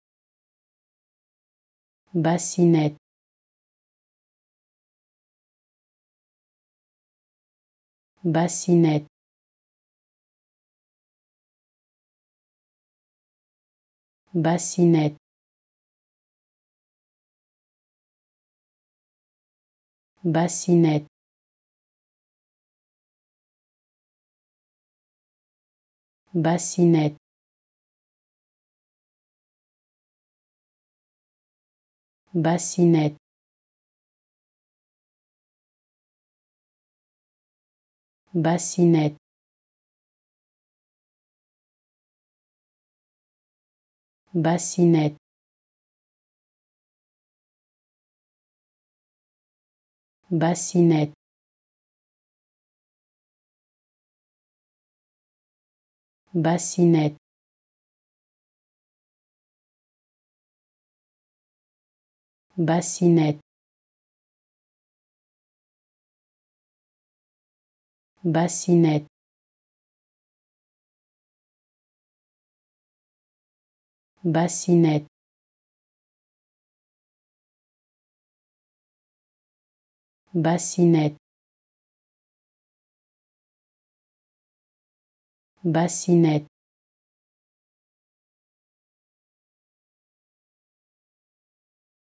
How to Pronounce ''Bassinette'' (Crib) Correctly in French
Learn how to say and properly pronounce ''Bassinette'' in French with this free pronunciation tutorial.